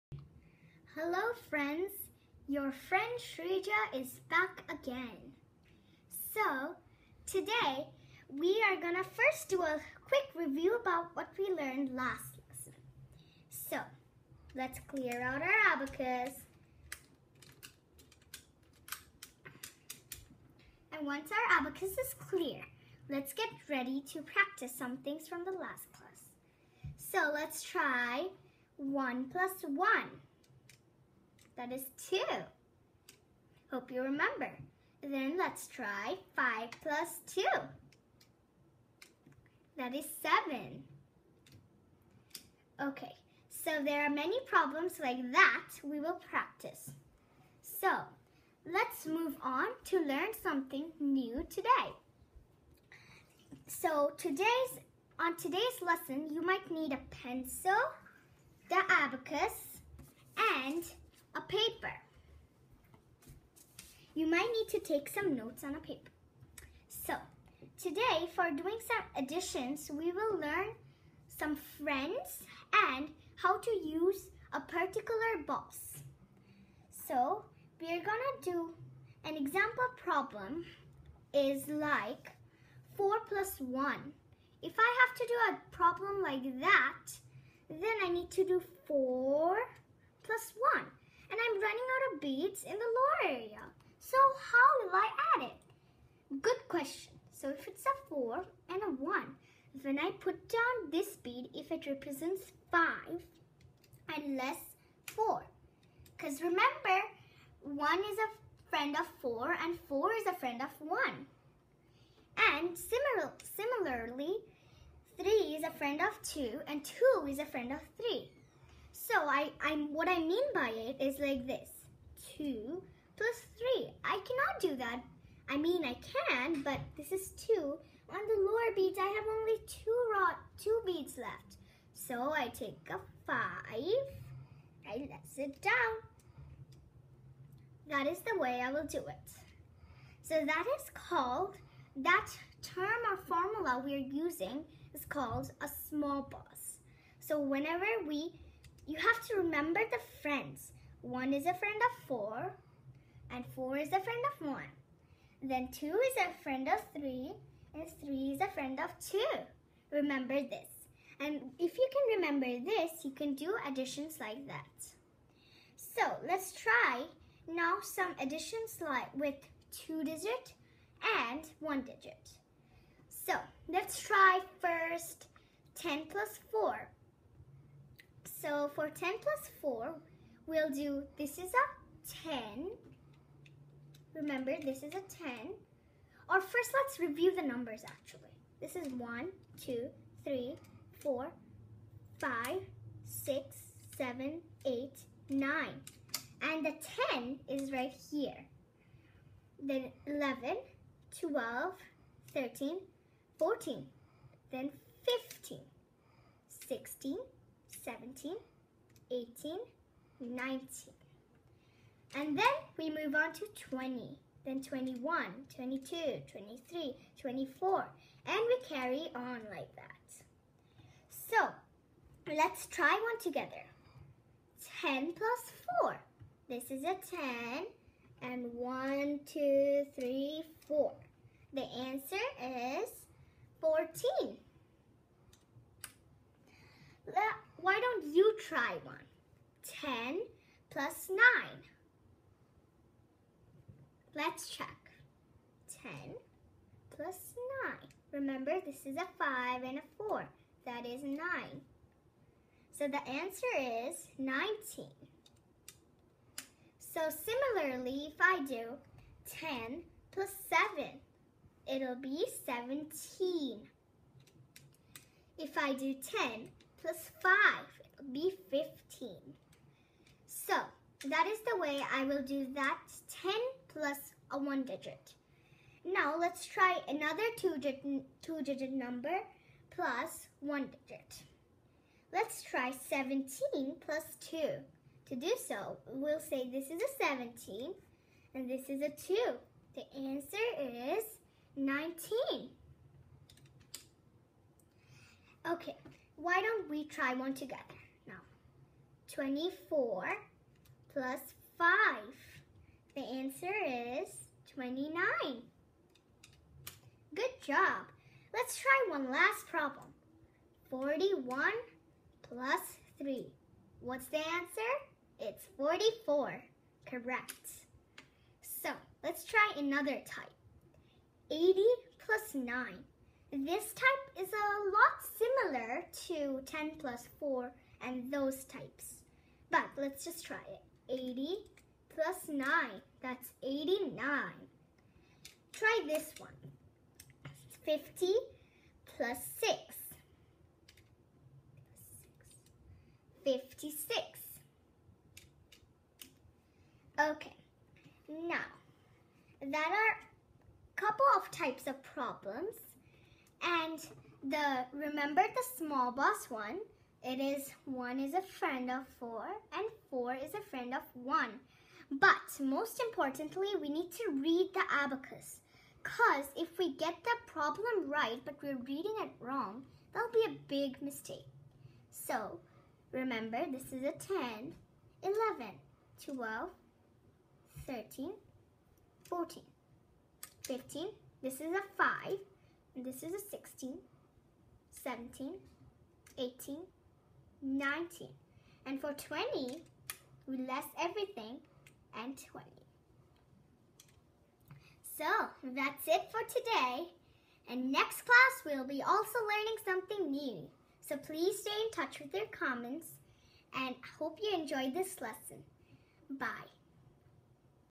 Abacus Lesson 3 Simple one digit addition by Shreeja (2018)
Japanese Abacus mental math tutorial lesson 3 addition involving TEN's place. Shreeja, a 9 year old designs her and teaches kids keeping in mind her own learning experience. Lessons are short, teaching one concept at a time